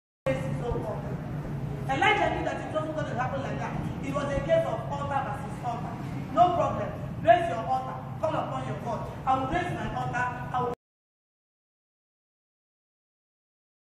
Benin, Nigeria - Inaugurations of F.G.F, A.K.A Faith Giants Forum(Part 3).
F.G.F, A.K.A Faith Giants Forum Presents: Benin - Inaugurations,  March 2012(Part 1). Host Evang. Mrs Uloma Ojei, Int'l President/Founder, FGF.